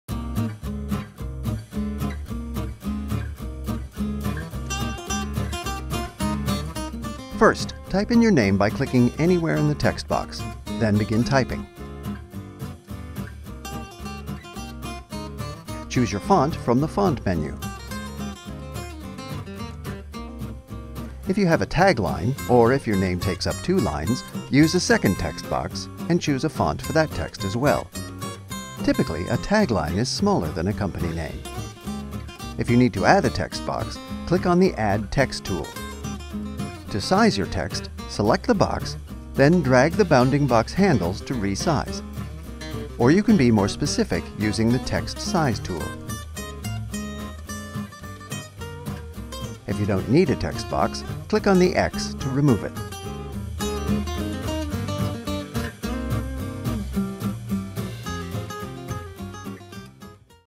Logo Maker Tutorial: Editing Text
This logo maker tutorial demonstrates how to edit and add text. Includes font selection, changing colors, resizing text, and adding logo effects.